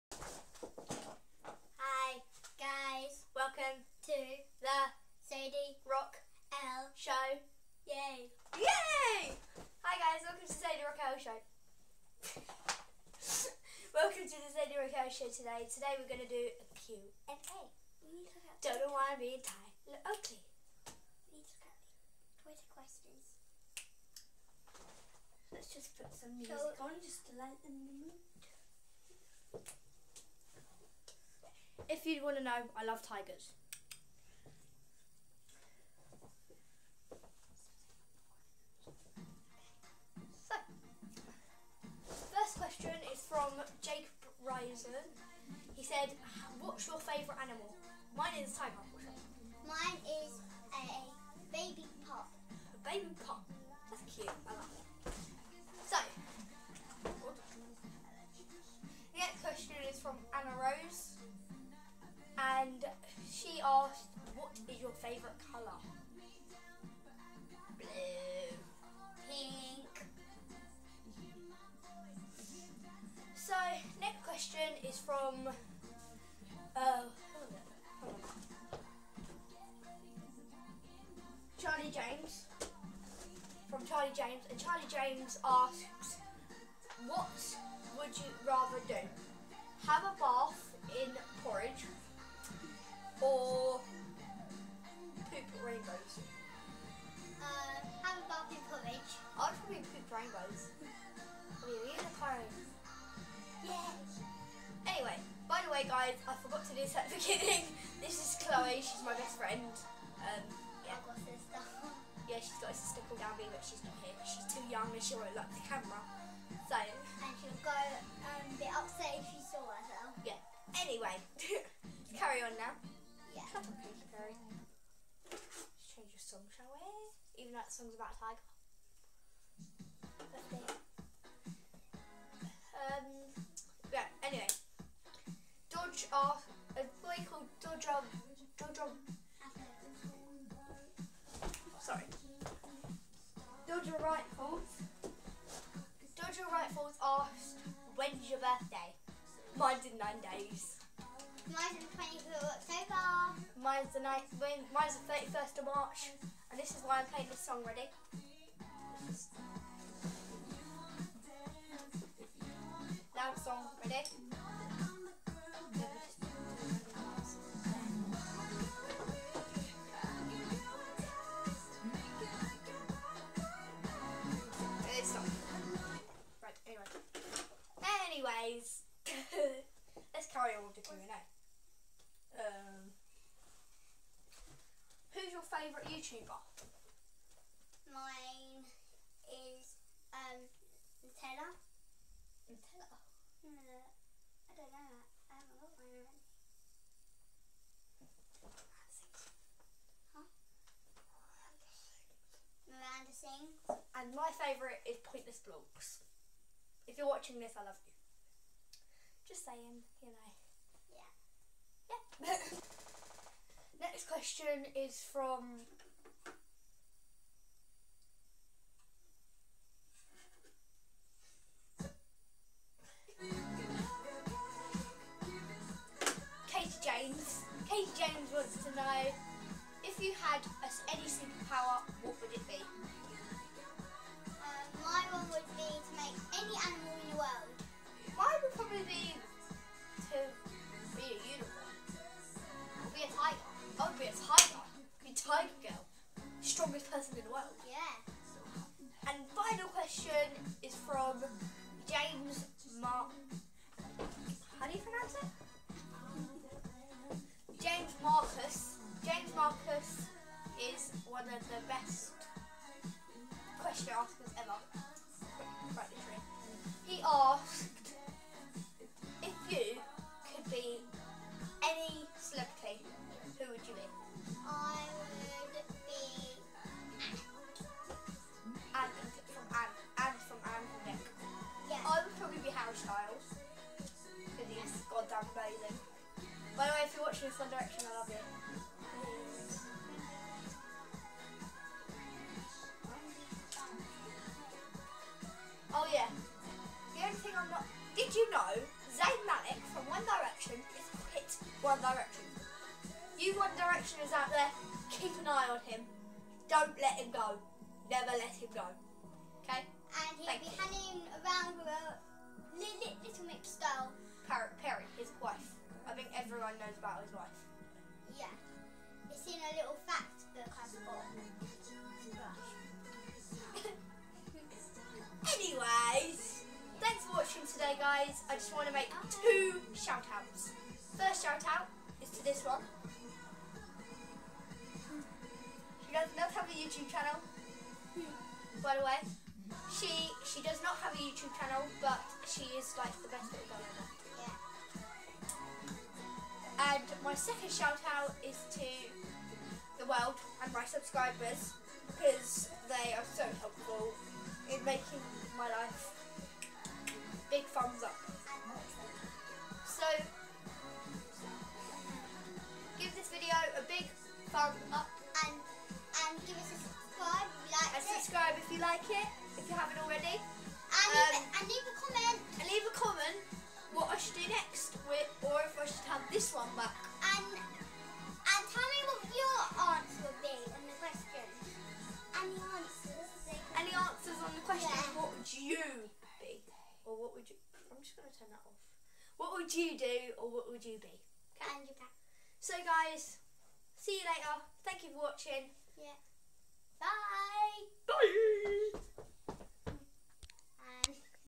Q&A with my friend Chloe /Sadie rockell show
Me and my bff made a video on a load of questions so we awnserd them YAY!